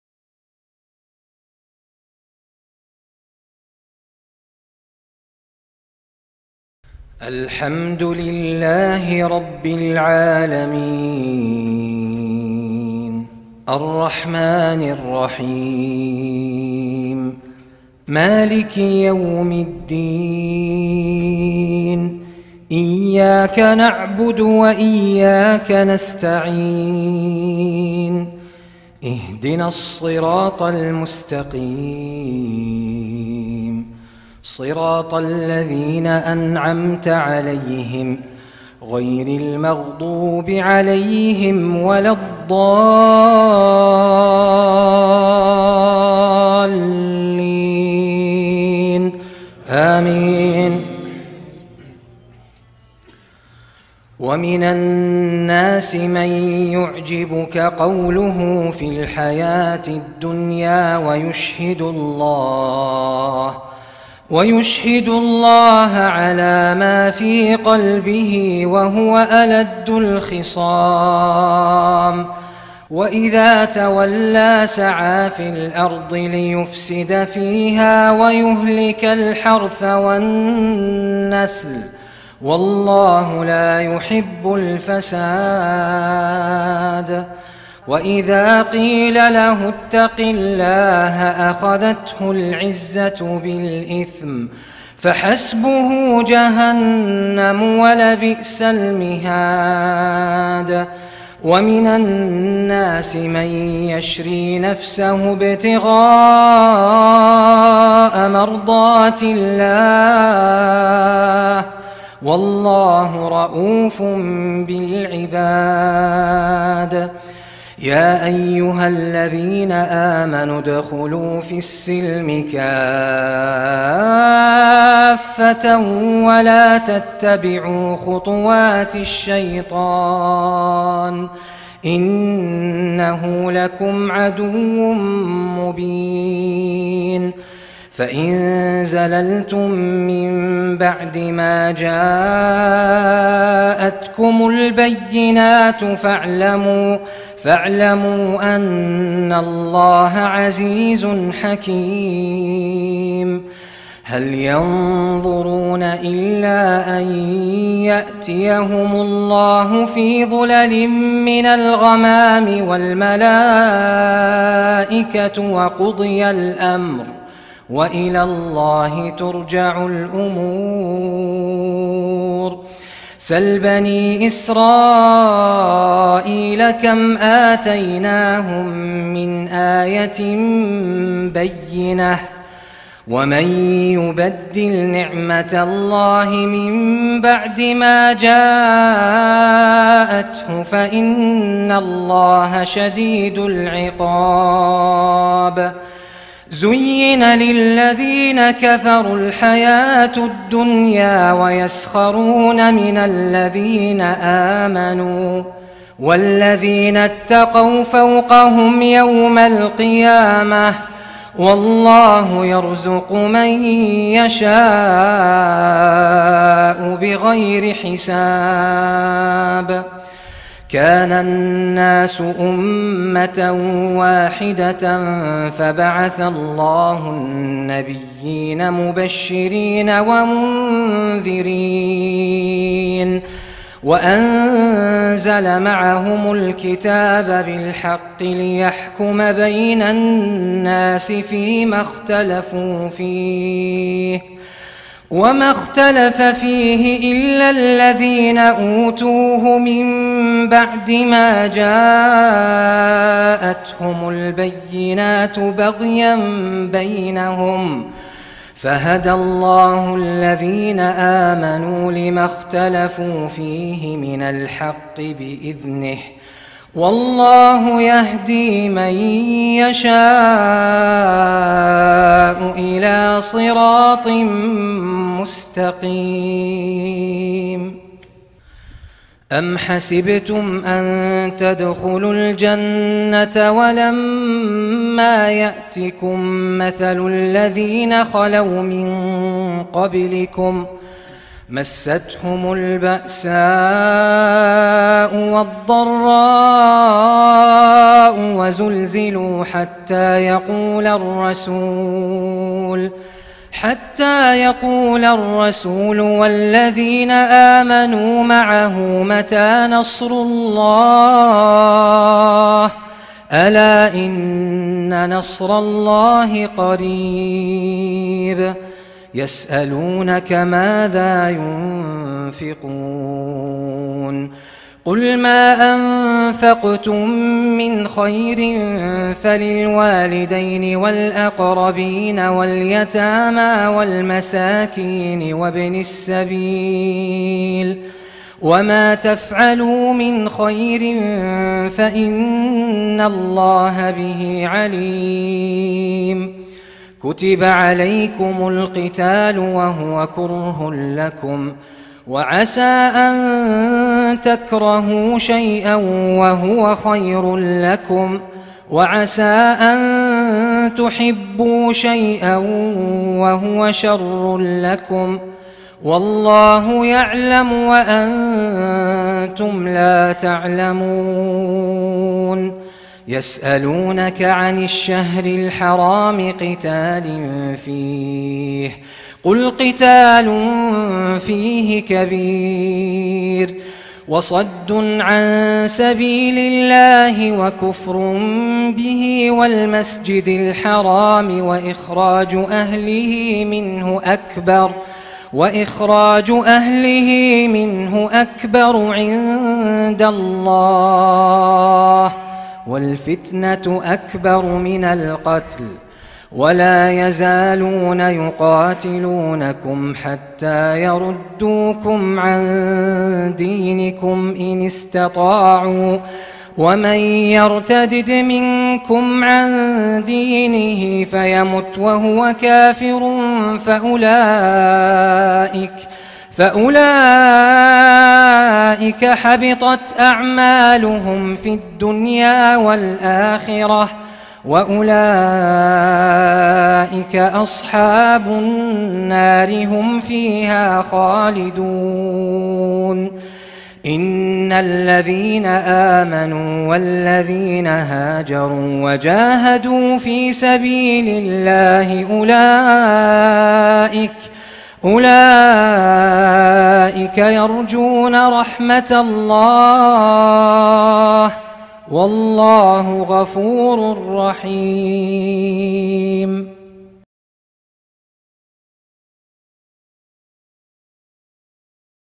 "Soothing Quran Recitation by Sh. Moutasem Al Hameedy 🌙📜"
Relax and rejuvenate with the soothing recitation of the Quran by Sh. Moutasem Al Hameedy. Let the melodious verses and gentle tones wash over you, bringing peace to your heart."